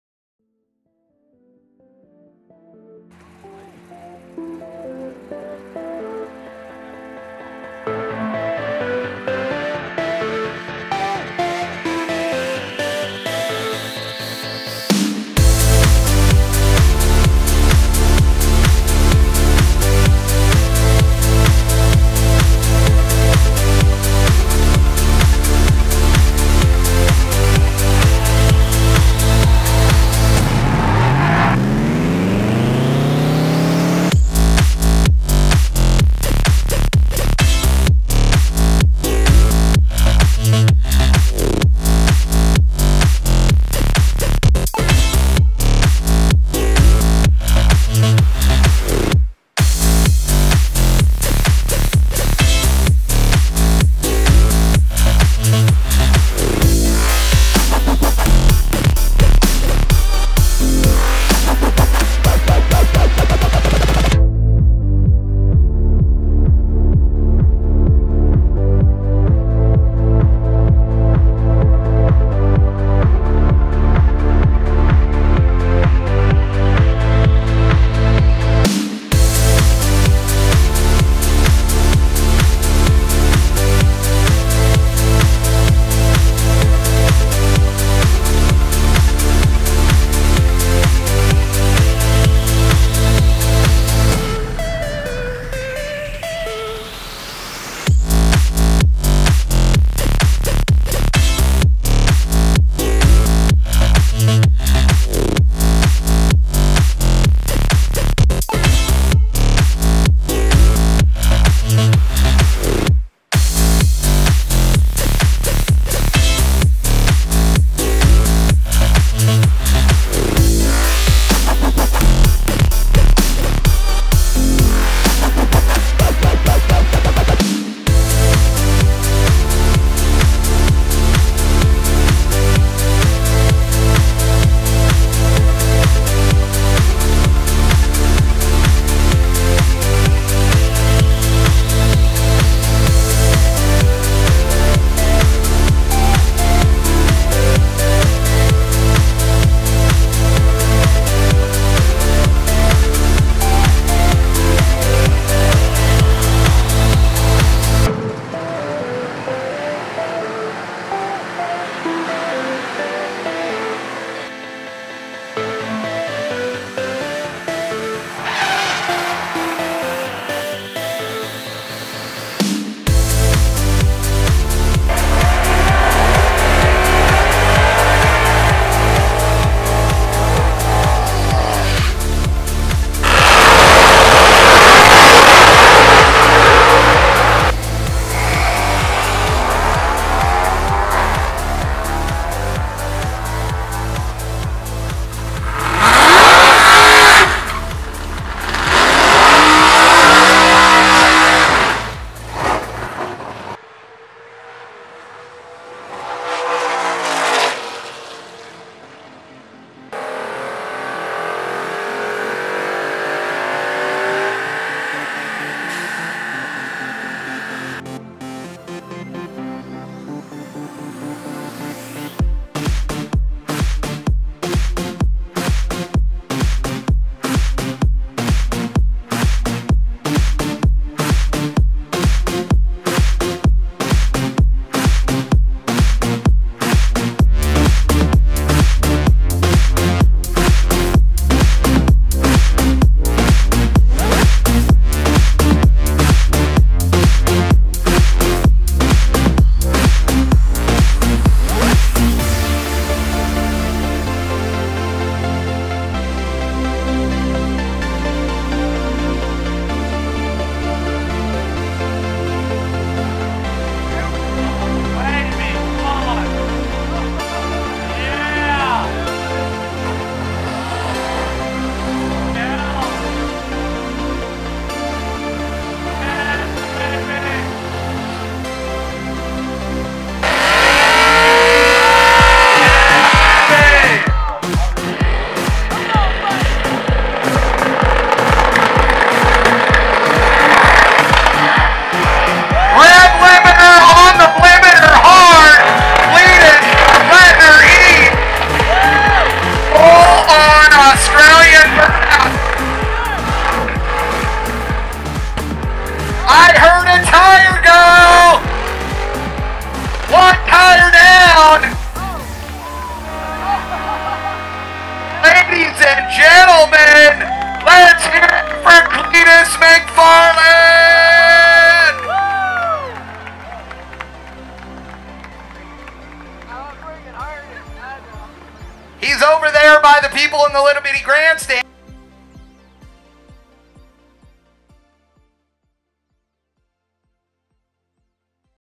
2019 Holley Ford Fest Highlight Video
Highlight video recap of the 2019 Holley Ford Festival in Bowling Green, KY at Beech Bend Raceway Park.  It was a great weekend meeting all the ford fans out there and watching the great NMRA drag racing, autocross, drifting, burnouts, car show and much more.